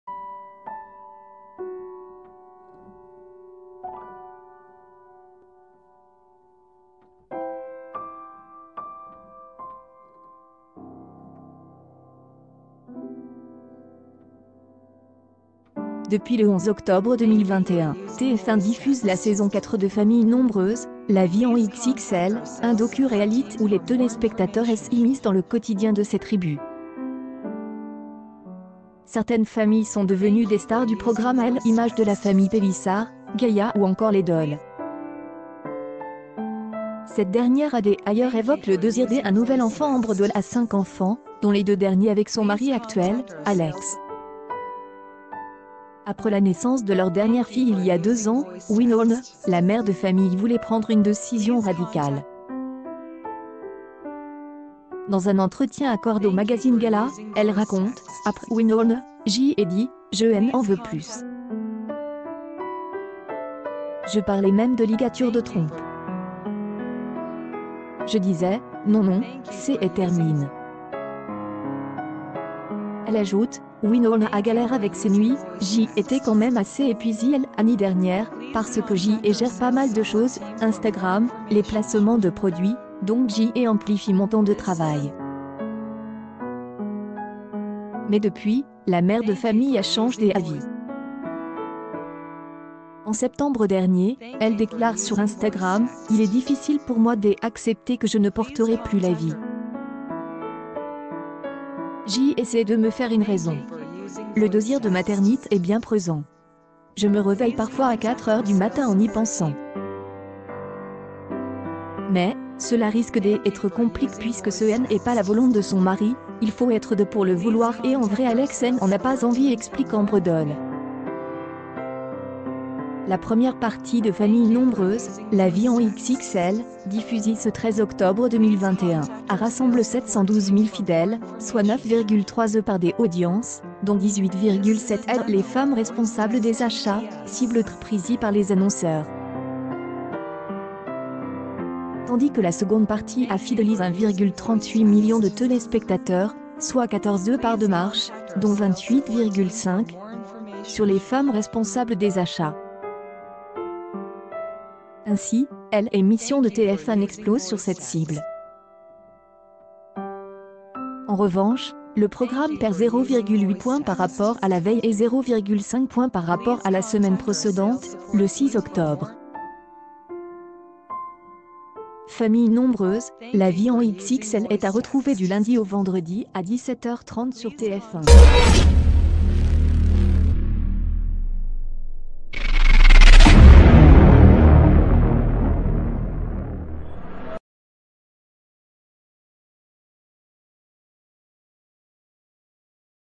Famille XXL : Ambre Dol évite une décision radicale, TF1 explose|toutelatele365
Famille XXL : Ambre Dol évite une décision radicale, TF1 explose|toutelatele365 Depuis le 11 octobre 2021, TF1 diffuse la saison 4 de Familles nombreuses : la vie en XXL , un docu-réalité où les t